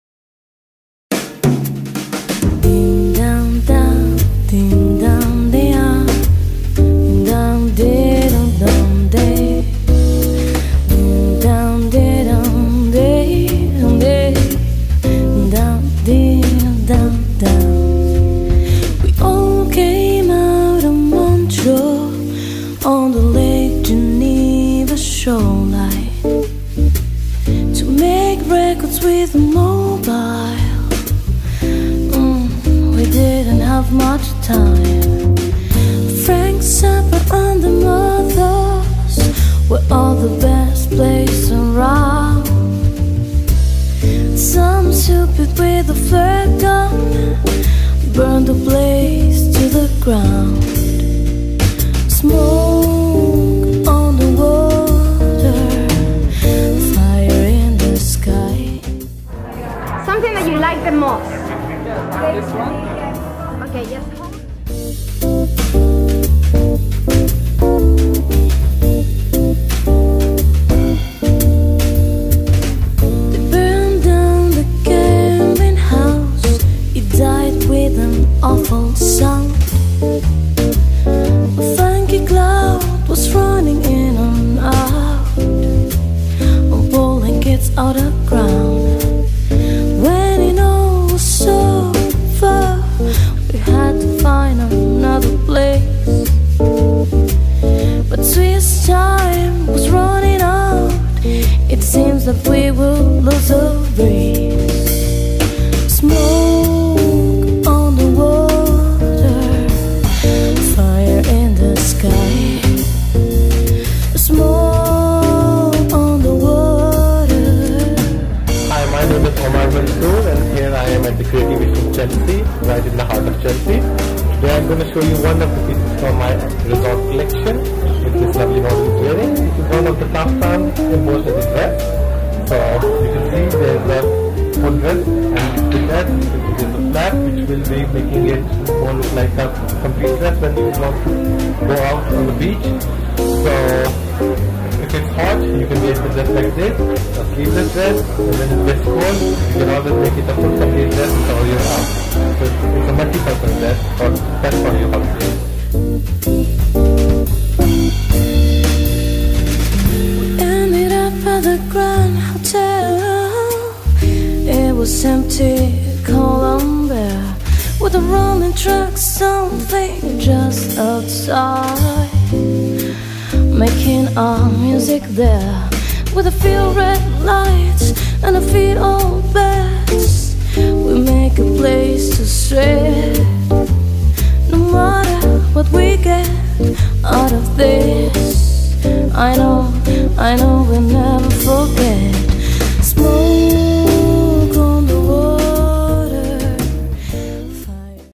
ART IN FUSION TV - ART & FASHION @ CREATIVATIONS-CHELSEA
CREATIVATIONS-CHELSEA
A REVOLUTIONARY CONCEPT BOUTIQUE/GALLERY
301 FULHAM ROAD -
REBECA RIOFRIO CEO OF ART IN FUSION HAD A VISIT TO THE ART & FASHION exhibition at the White Space Gallery - CREATIVATIONS-CHELSEA
Had a warm welcome by the gallery owner Madam Laura Jamieson
, a lovely space in Fulham Road. We had the pleasure to meet various painters and designers among them Apu Jan and Omar Mansoor